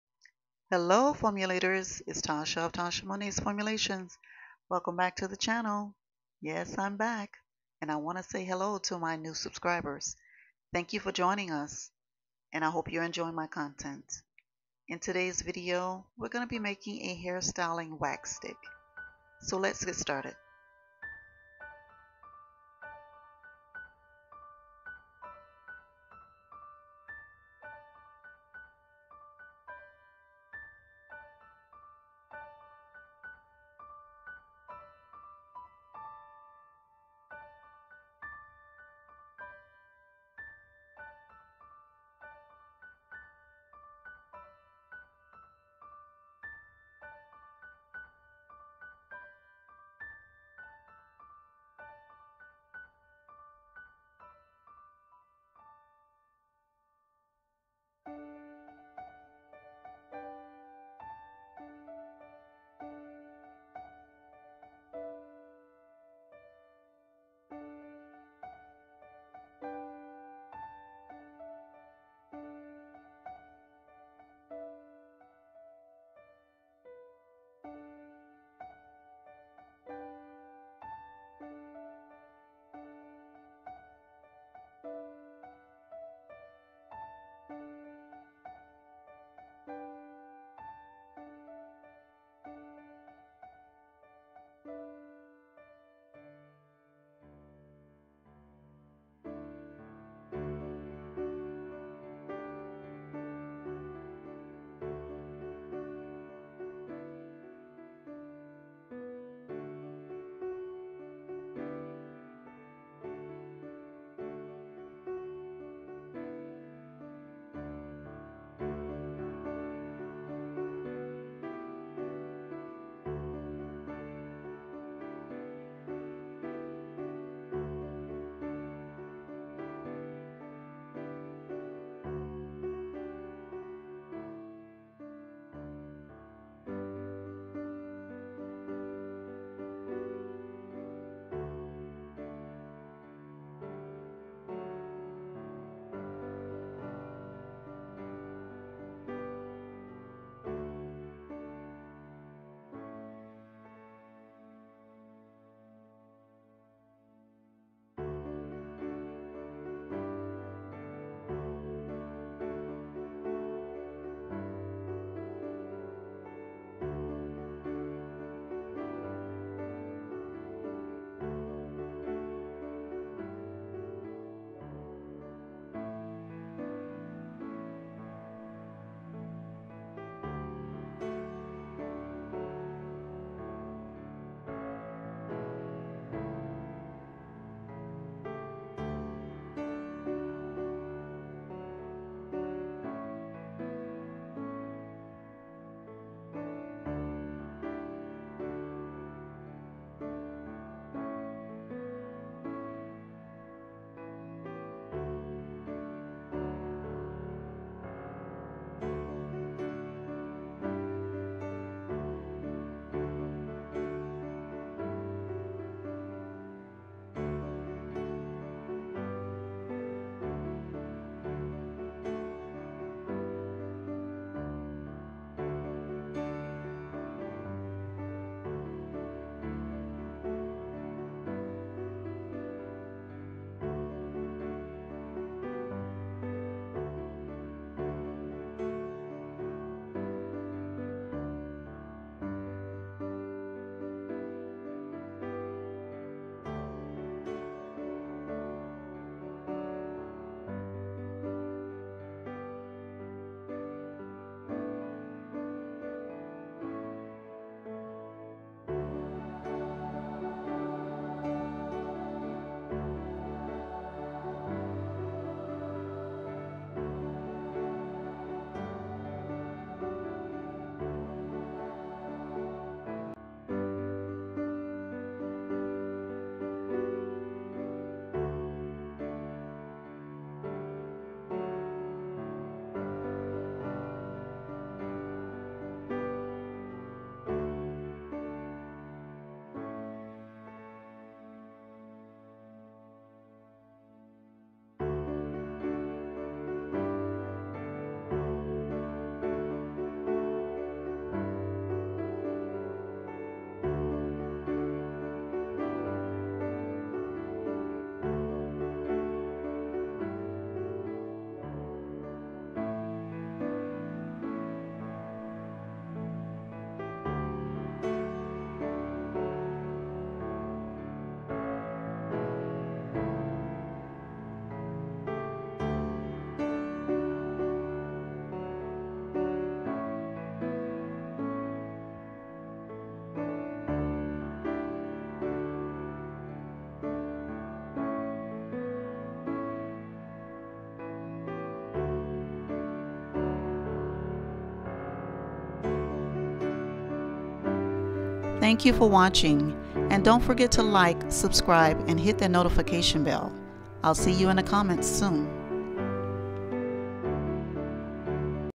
HOW TO Make a VEGAN HAIRWAX
Lay those edges, tame those fly-aways and that "kitchen" with this styling hair wax. It will control and hold your style without stiffness. Each ingredient serves a purpose to condition soften and help keep hair moisturized.